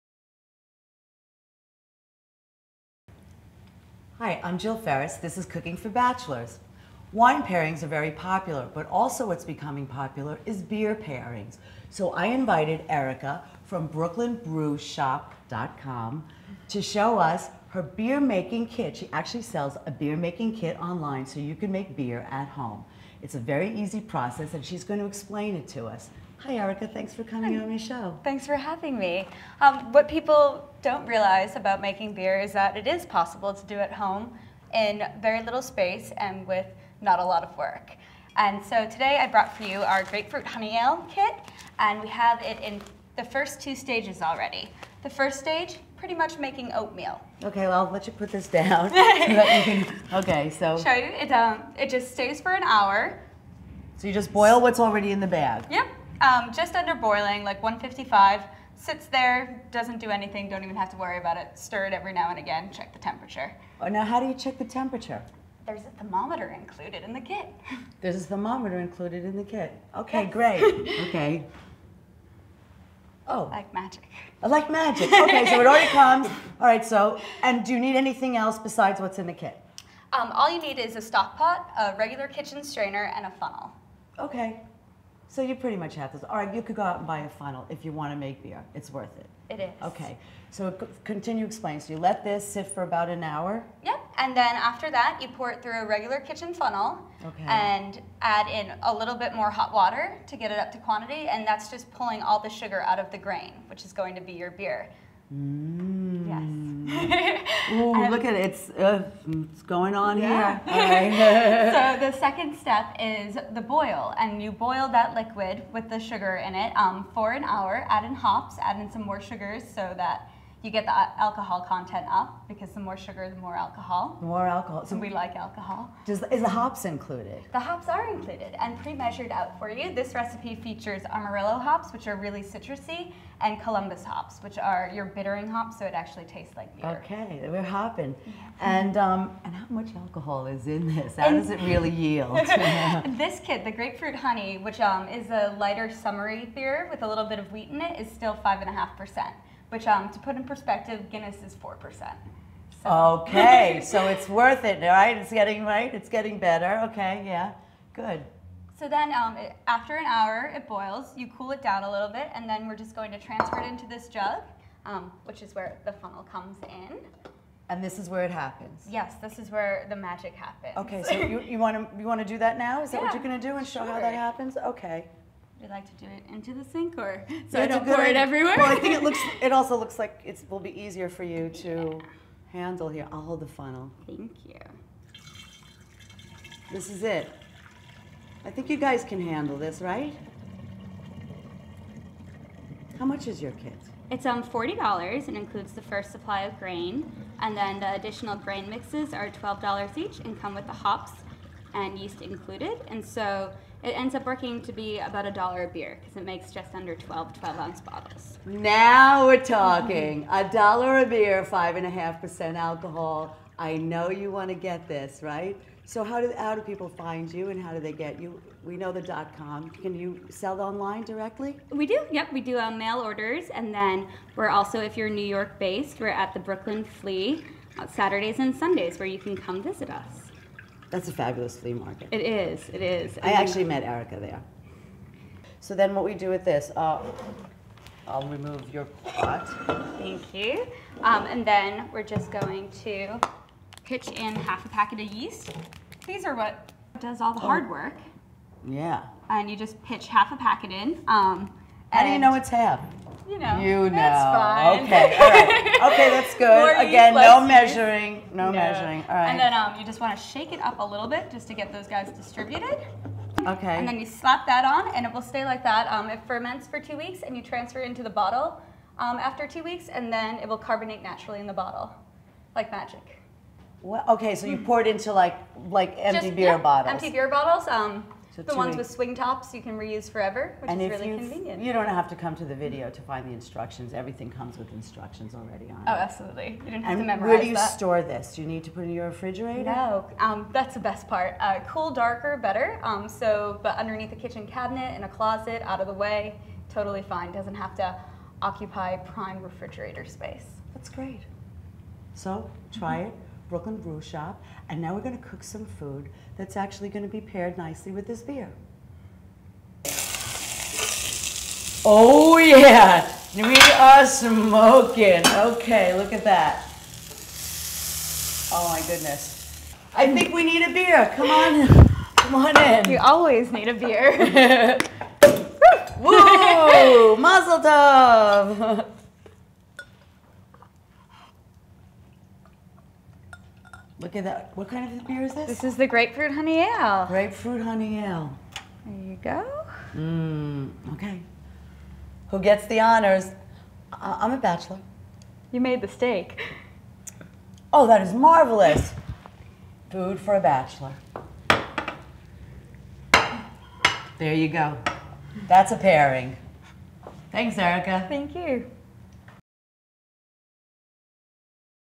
Beer Making by Cooking for Bachelors® TV
Erica from Brooklyn Brew Shop shows us how easy it is to make beer at home with one of their kits which can be purchased online at their site or at the Brooklyn Flea. It takes about an hour from start to bottle. Then the bottles sit for two weeks and the result is great beer for a great price. I decided to pair the Grapefruit Honey Ale with a grilled ribeye steak, sauteed onions and collard greens. When you cook for yourself you can Save Money, Eat Healthier, Live Greener and Have Fun!